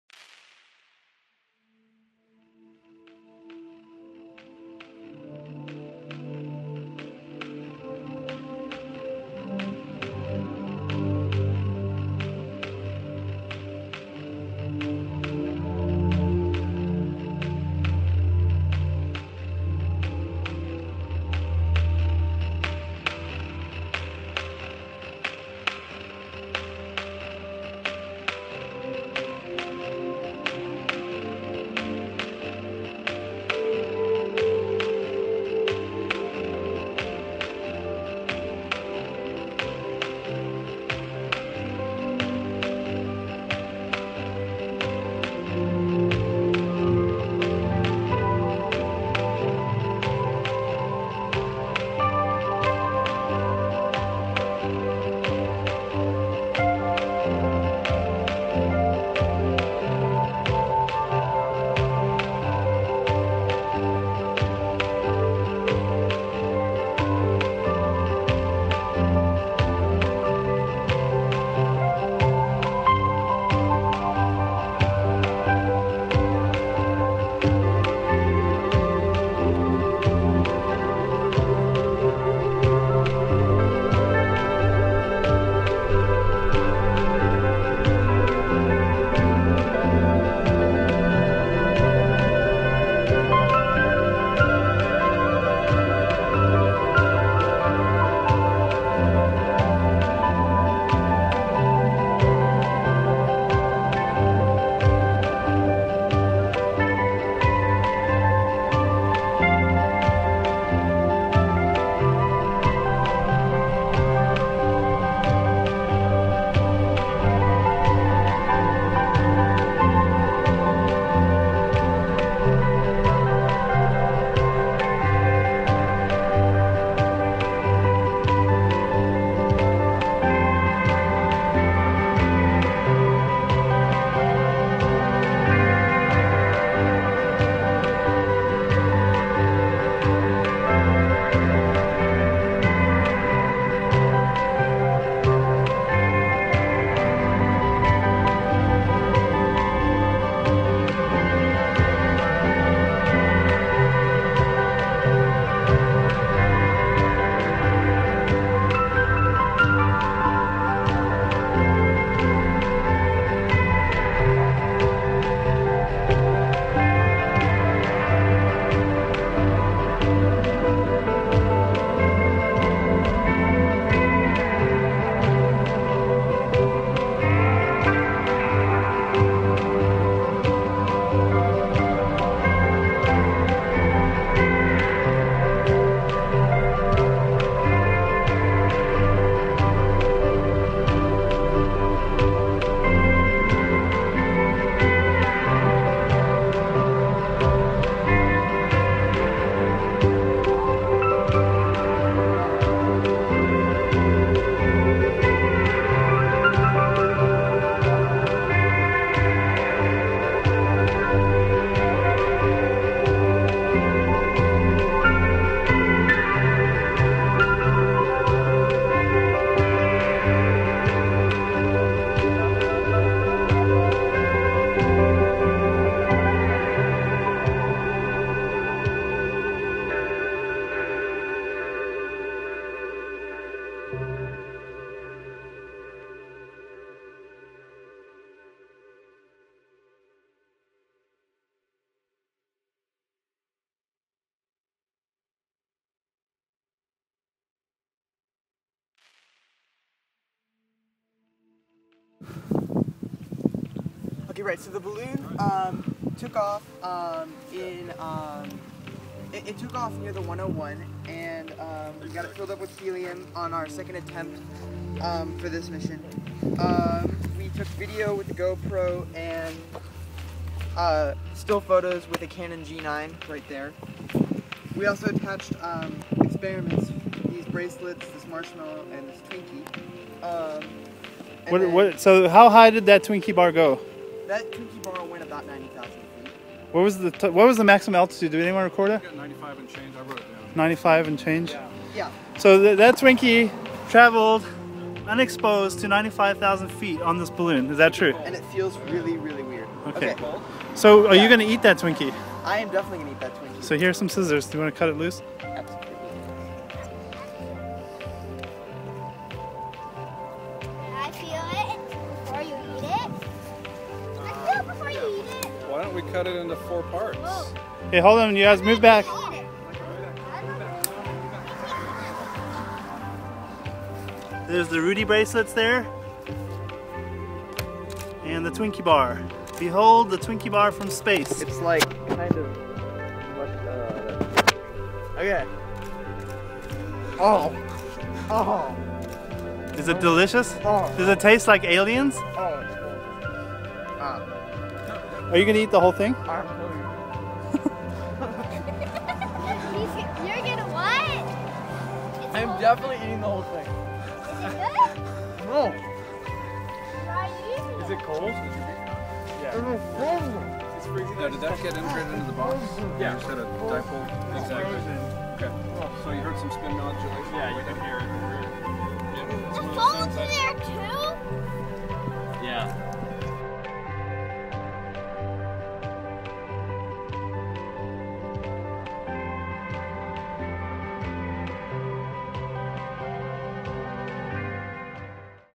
Kid eats space Twinkie - High altitude balloon project
Geoff (14) and I spent over 8 months building and planning this flight.  Geoff studied for and passed the FCC exam to operate a ham radio station so that we could track the balloon as it flew.
One of the fun things Geoff wanted to test was what would happen to a Twinkie after a wild ride over 3x as high as the airlines fly :)
So we sent an unwrapped Twinkie up to an altitude of 95 thousand feet above the earth strapped to a weather balloon - and then Geoff ate it.
In addition to the Twinkie we sent up a marshmallow (which swelled as the pressure dropped and according to the kids tasted "normal")  some cameras (a Go Pro HD and Canon G9 running CHDK) and temperature sensors connected to an Arduino...
Between the Twinkie and the marshmallow are some Rudy bracelets in honor of our sweet little friend Rudy

for photos and graphs of all the data collected.

Original music written and performed by the amazing and talented Tad Wagner and available for download:

Unfortunately for us, both cards on the Go Pro and Canon G9 filled up before the burst :( The highest photos we got are from around 80K feet AGL.

Real time location / altitude tracking was done with a tiny ham radio APRS transmitter connected to a GPS.